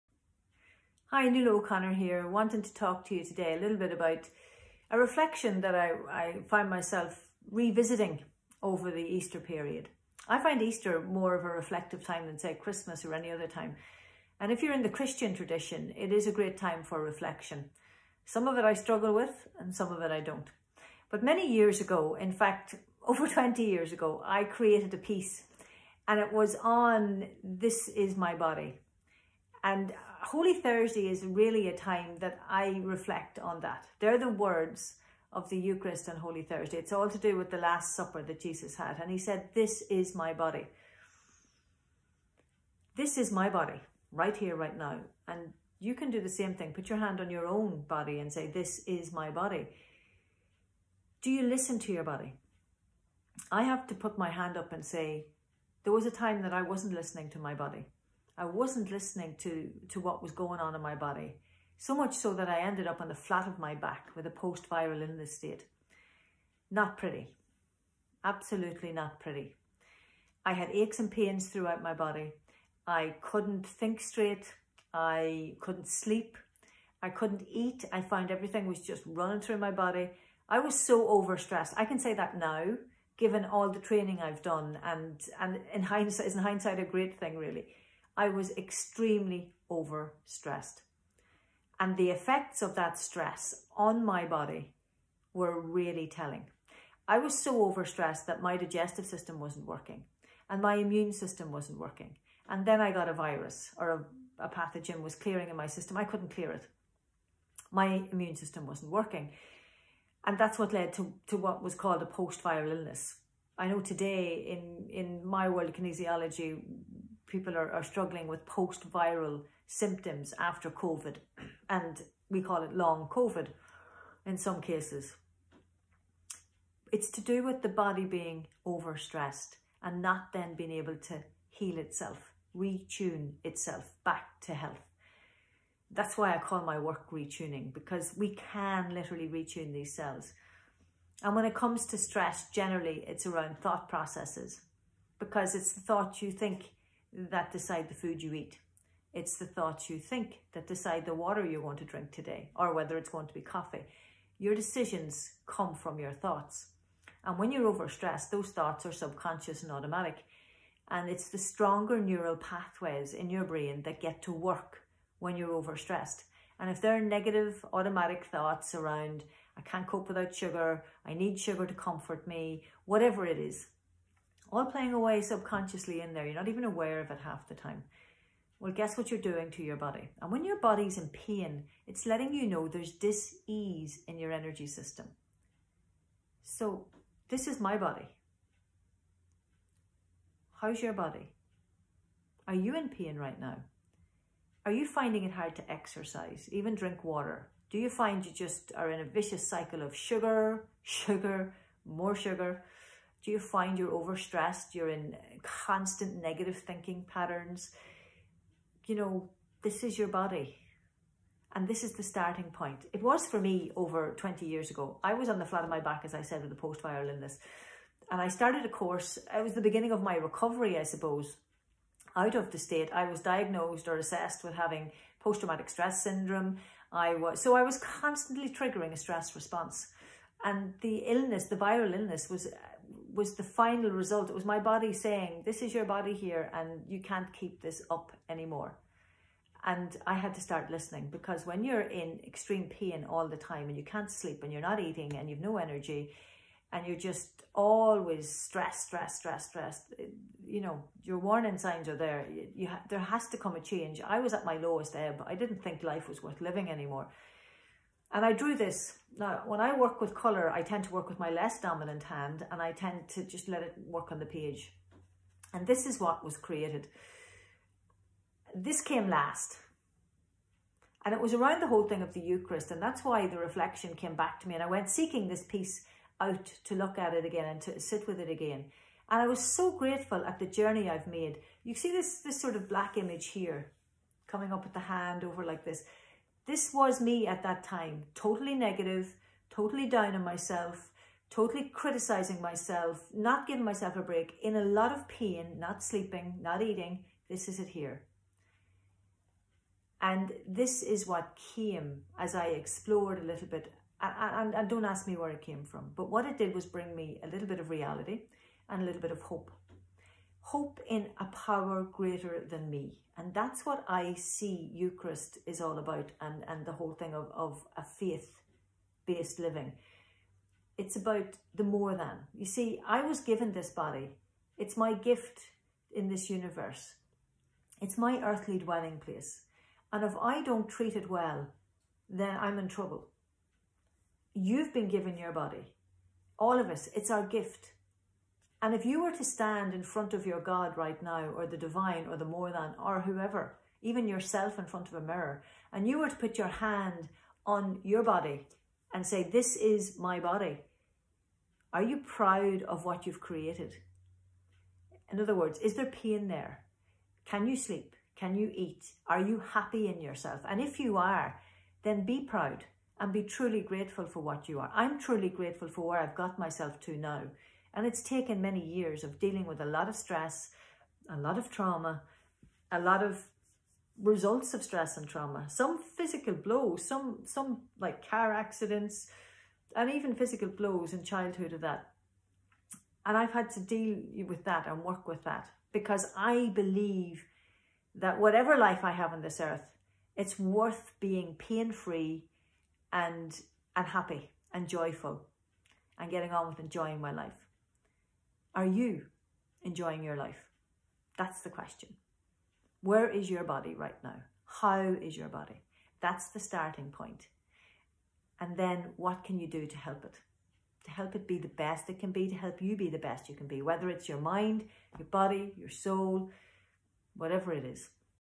This Is My Body - How Are You With Your Body
Life can throw a lot of challenges and stresses at you. It has at me. your body tells the tale of all it has been through. What tale does your body tell.

I had an amazing soul experience many years ago as I was reflecting on the Easter experience of 'This is my body'.